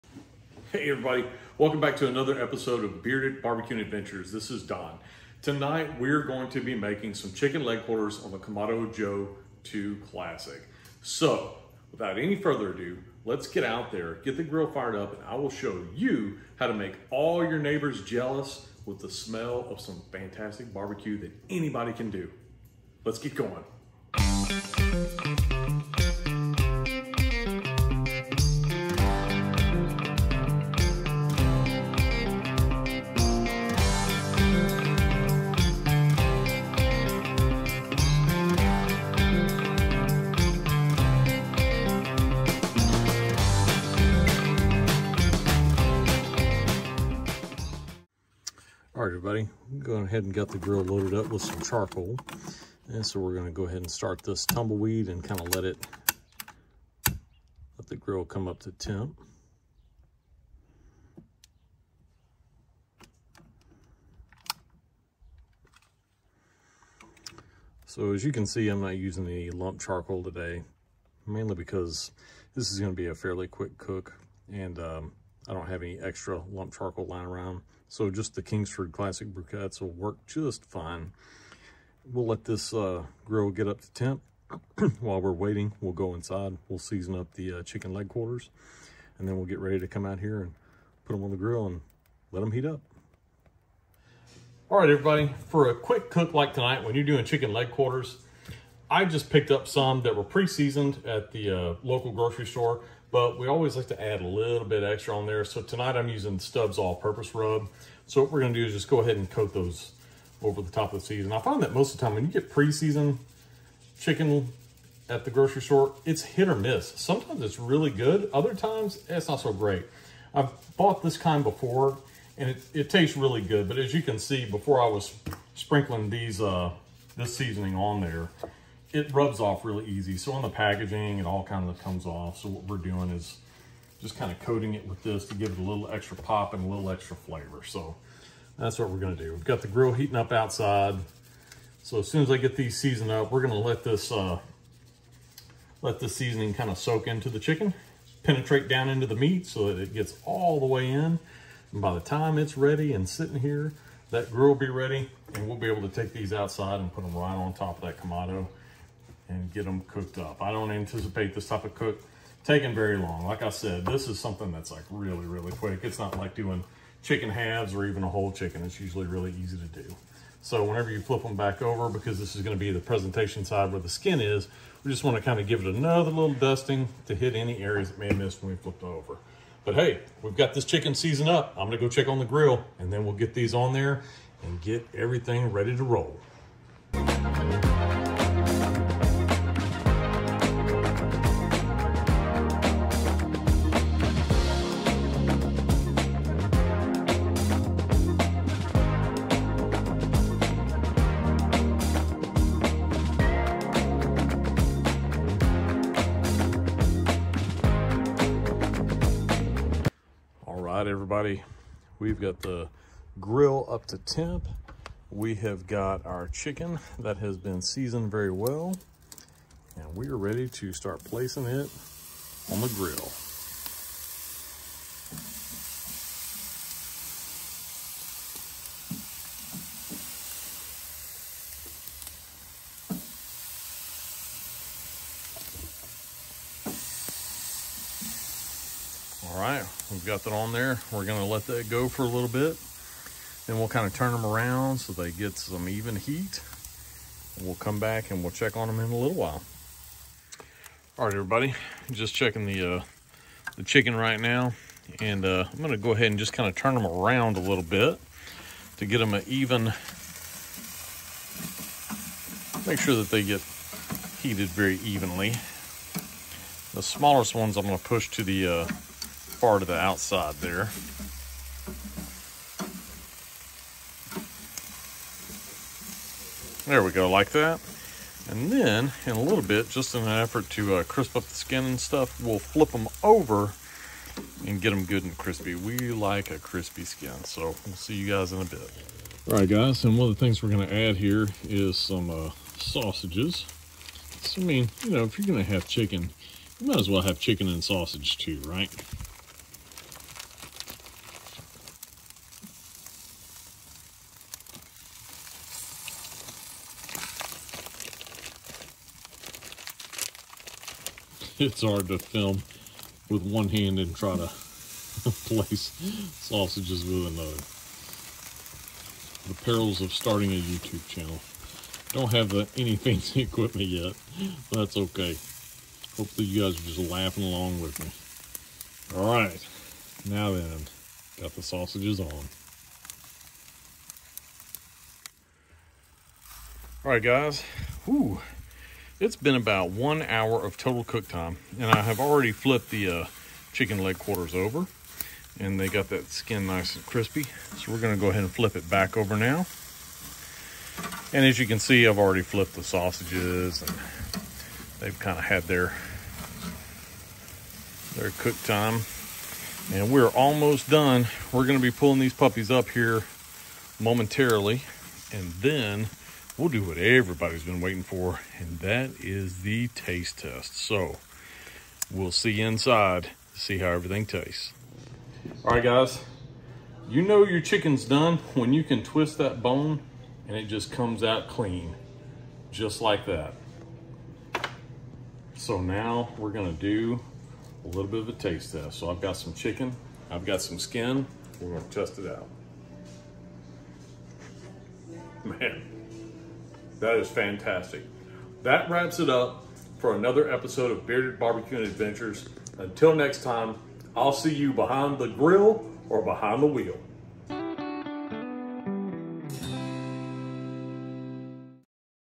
Grill Like a Pro: Perfect Chicken Quarters
Join Don on another epic episode of Bearded BBQ and Adventures as he shows you how to grill like a pro and make perfect chicken leg quarters on the Kamado Joe II Classic. Get ready to make your neighbors jealous with the mouthwatering smell of fantastic barbecue that anyone can master.